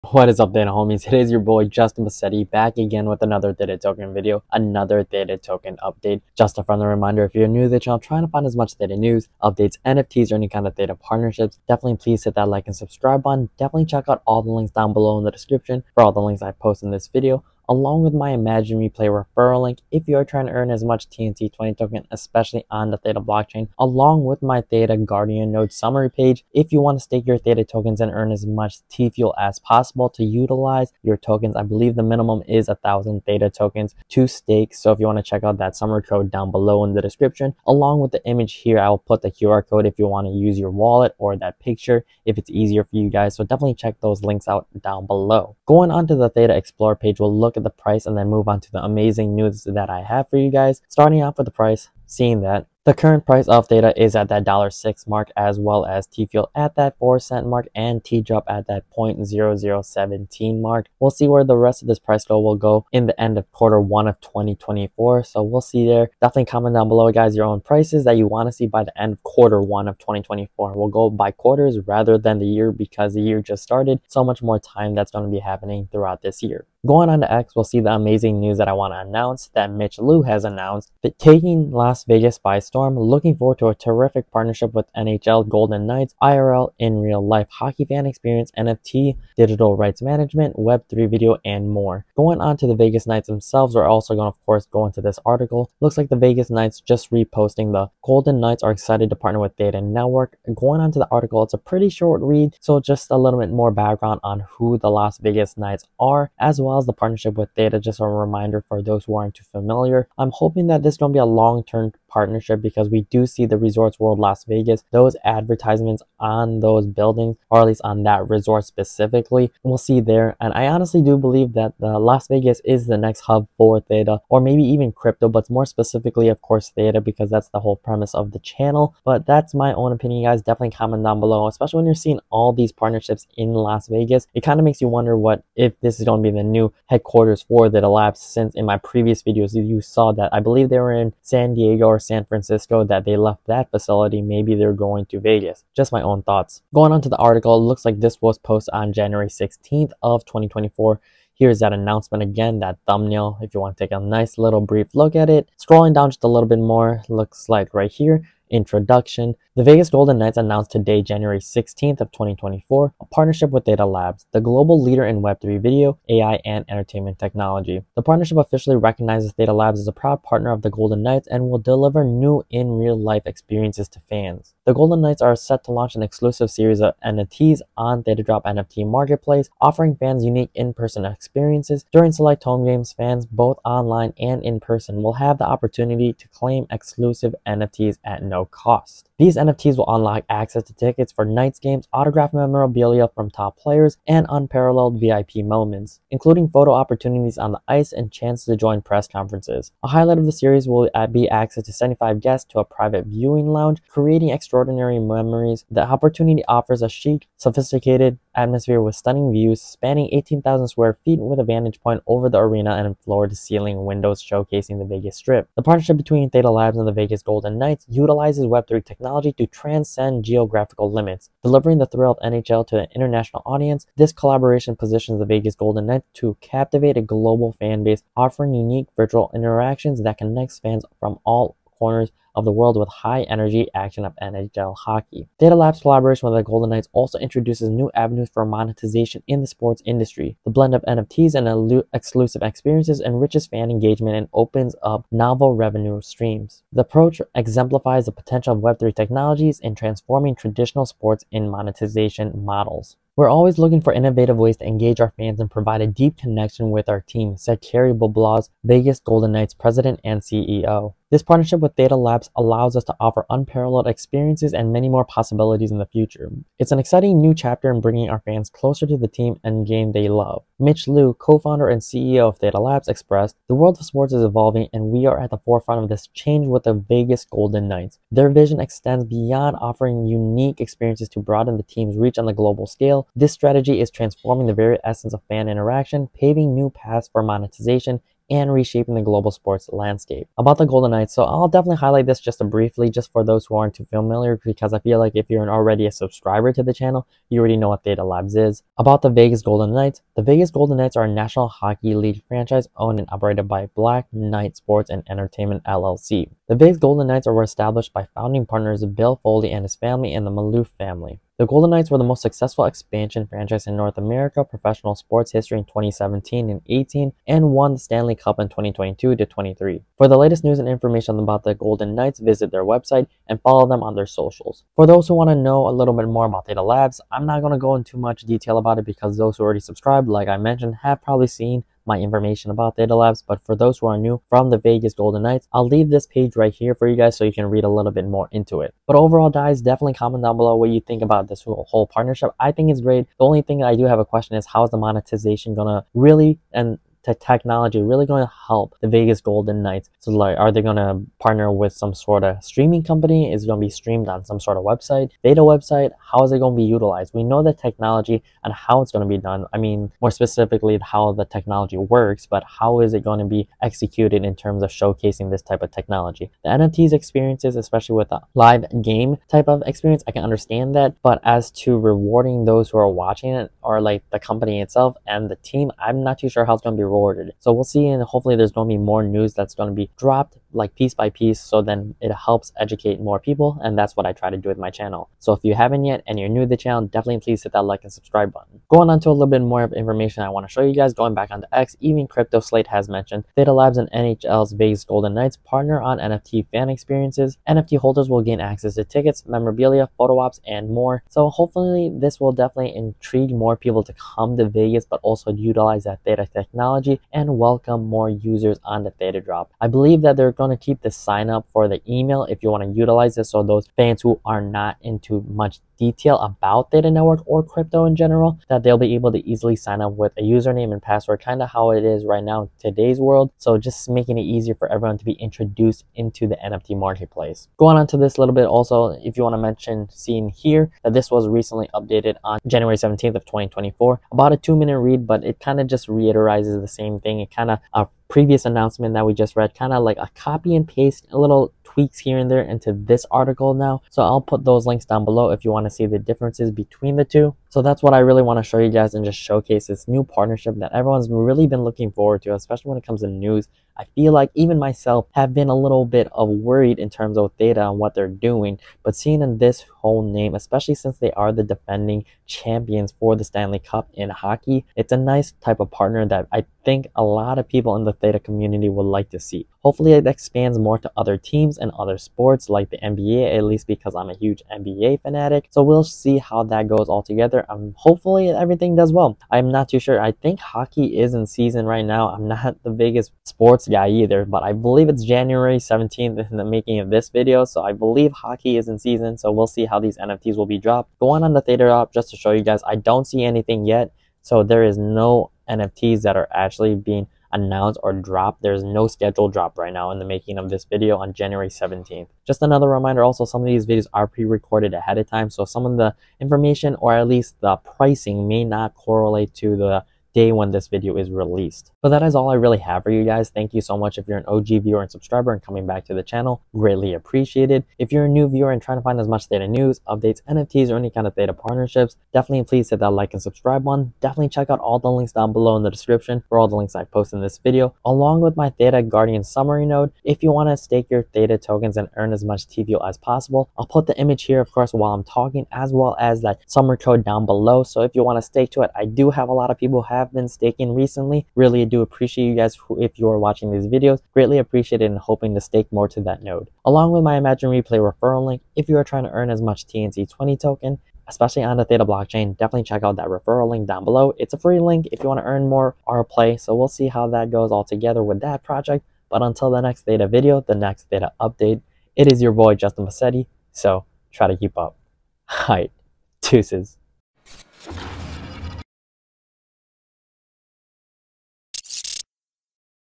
THETA NETWORK TAKING OVER LAS VEGAS?! | THETA TOKEN UPDATES!
In this video, I breakdown the recent information that highlights the Las Vegas @NHL Golden Knights and their established partnership with Theta Network! I discuss the details of the partnership and where this could take us!

Theta Guardian Node:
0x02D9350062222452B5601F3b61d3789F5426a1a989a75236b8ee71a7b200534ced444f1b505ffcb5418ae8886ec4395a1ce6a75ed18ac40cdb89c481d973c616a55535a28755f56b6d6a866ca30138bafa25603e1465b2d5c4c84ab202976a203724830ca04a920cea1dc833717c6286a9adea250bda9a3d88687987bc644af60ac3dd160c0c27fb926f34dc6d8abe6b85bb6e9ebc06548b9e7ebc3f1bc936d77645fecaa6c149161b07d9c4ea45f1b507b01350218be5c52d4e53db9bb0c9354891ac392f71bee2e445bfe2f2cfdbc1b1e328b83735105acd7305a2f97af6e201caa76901